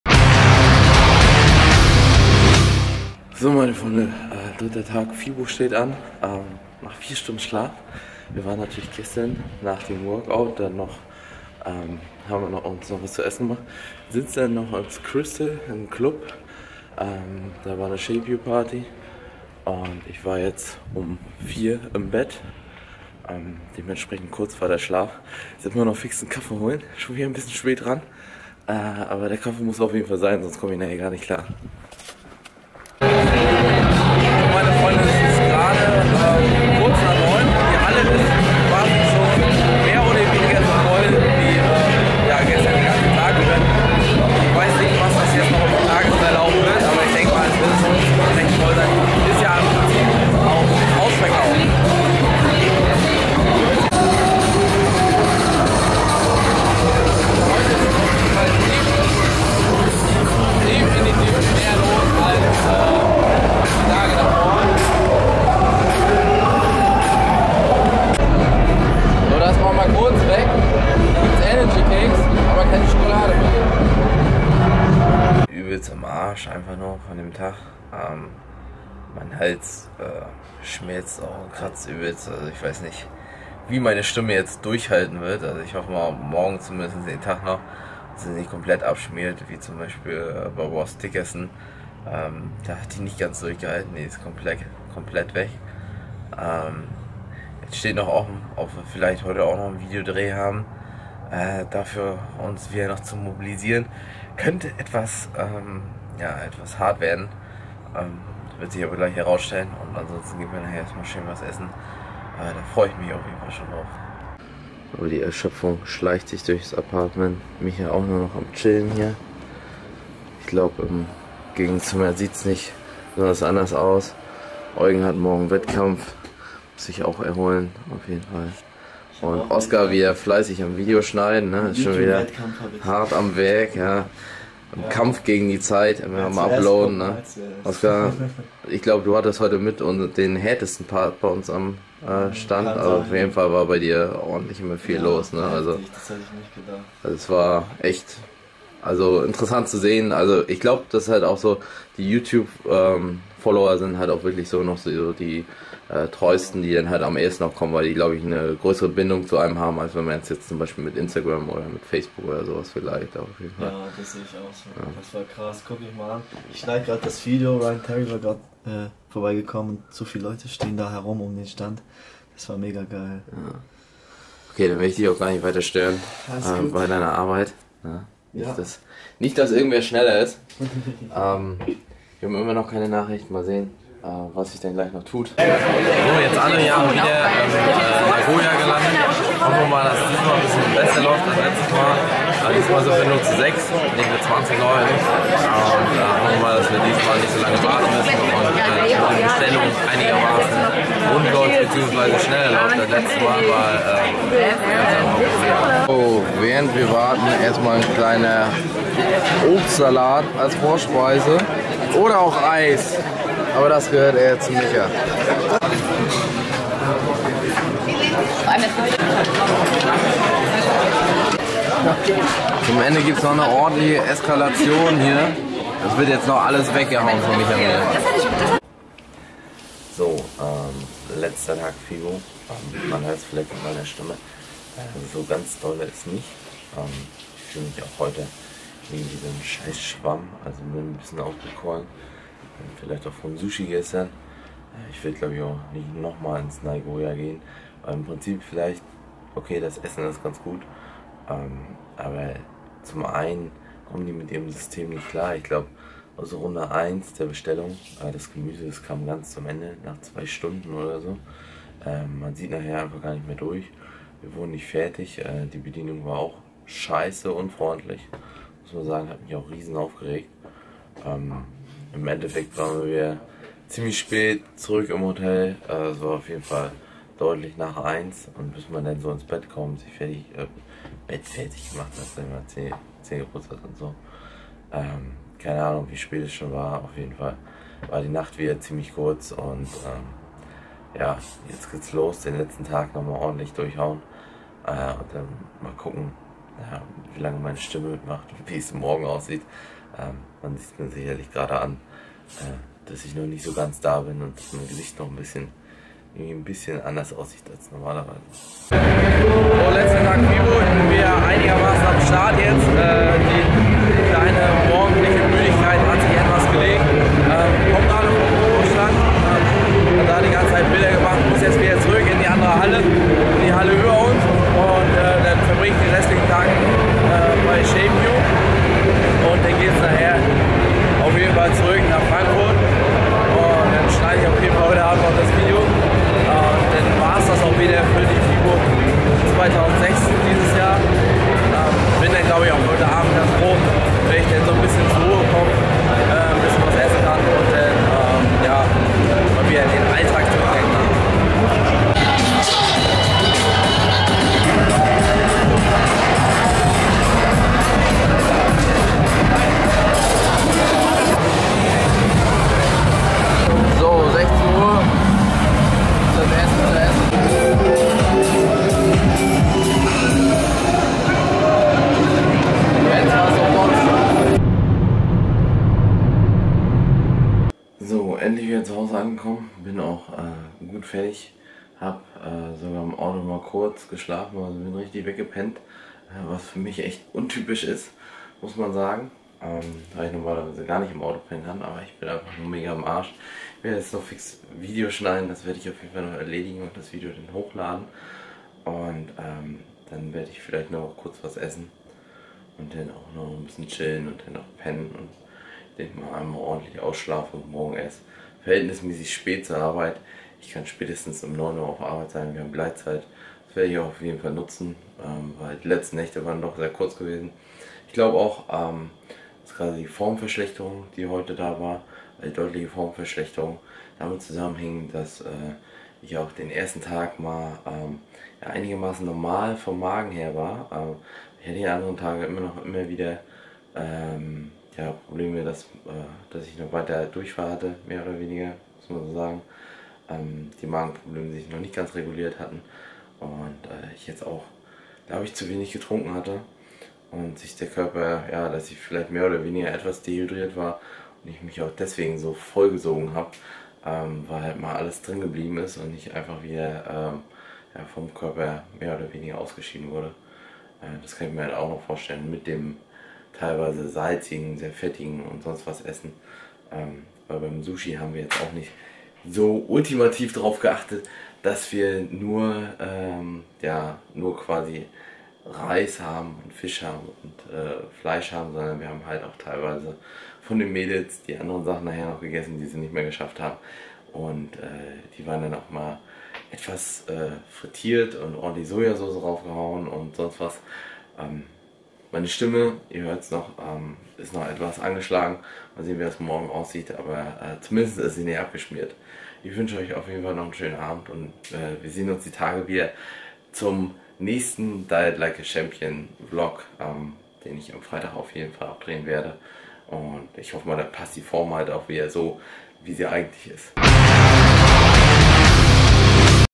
FIBO 2016 - VLOG SPECIAL - SAMSTAG UND SONNTAG
Der Fibo 2016 Samstag und Sonntag in einem VLOG geballt. Ein hartes, aber auch geiles Wochenende :D